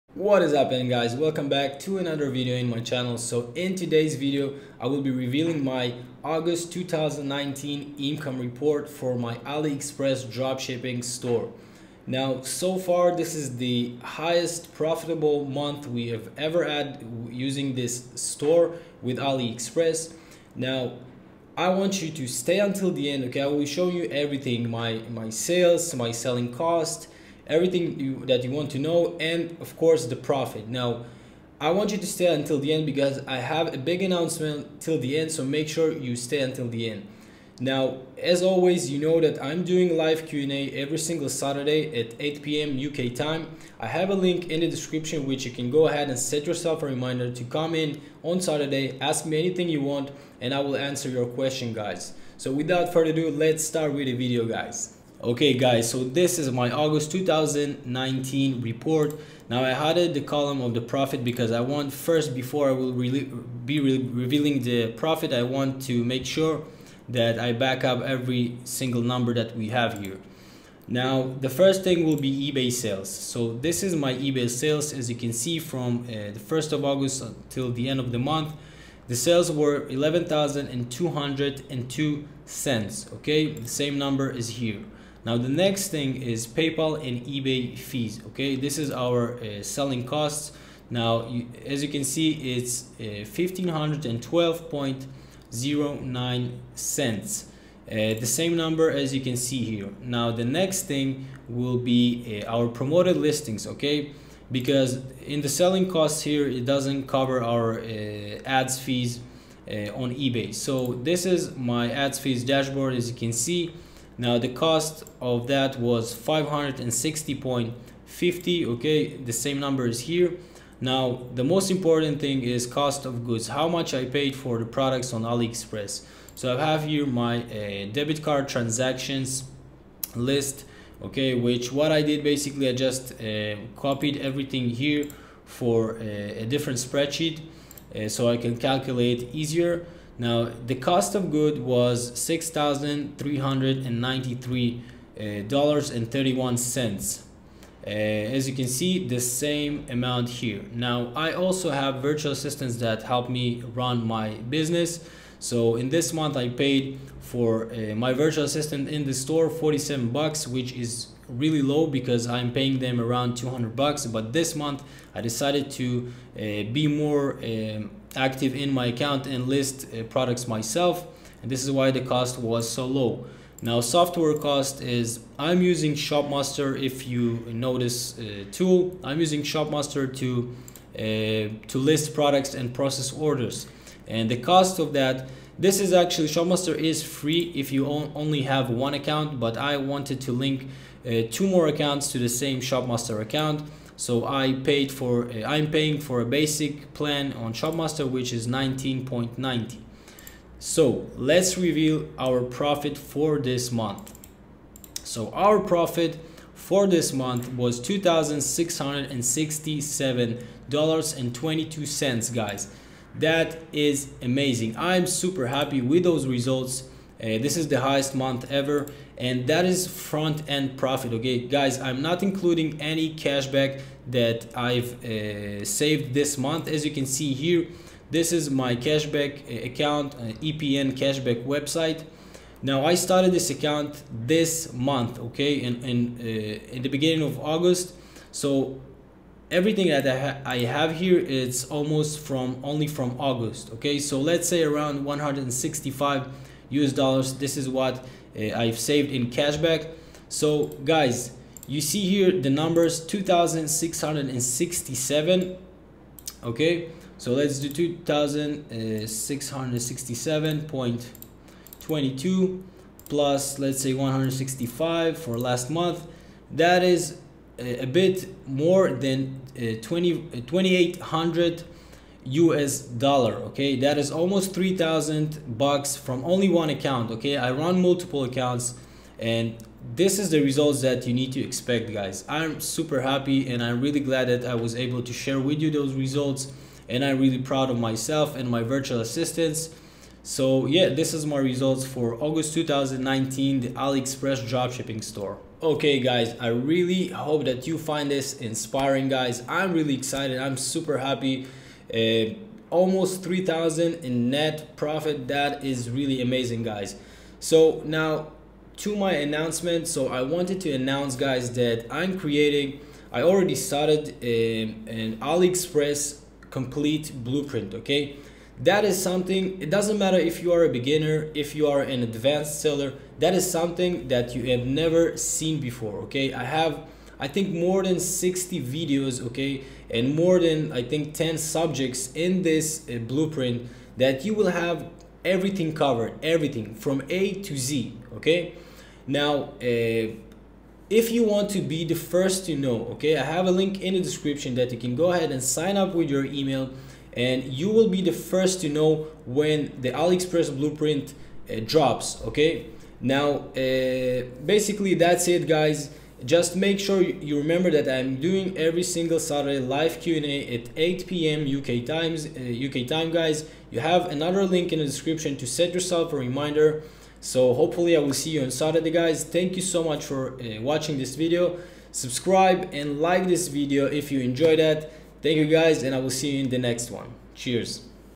Dropshipping From Aliexpress To eBay - August 2019 Income Report
In this video, I am showing my Aliexpress to eBay dropshipping store results. Dropshipping on eBay is a challenging thing, to make sales is not that hard but making a profit that is the REAL challenge. Around a year I discovered Aliexpress to eBay dropshipping and I started to see results really fast. Because of the cheap prices in China, I discovered that you can dropship from Chinese suppliers and make 20-30-40% profit margins and as I said, we are here to make a profit, not only sales!